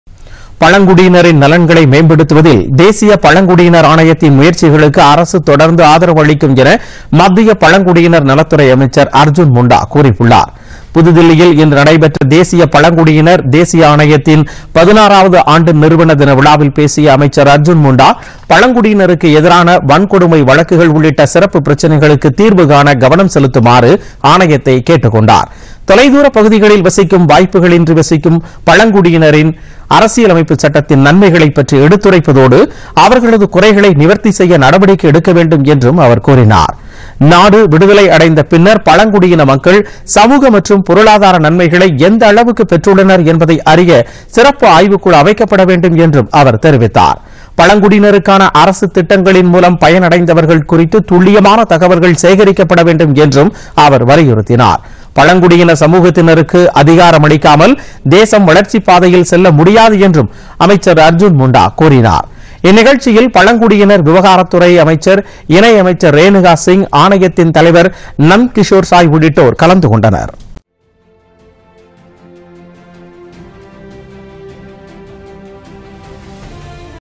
பழங்குடியினர் நலன் மேம்பாடு
பழங்குடியினரின் நலன்களை மேம்படுத்துவதில் தேசிய பழங்குடியினர் ஆணையத்தின் முயற்சிகளுக்கு அரசு தொடர்ந்து ஆதரவு அளிக்கும்  - மத்திய  அமைச்சர் அர்ஜுன் முண்டா .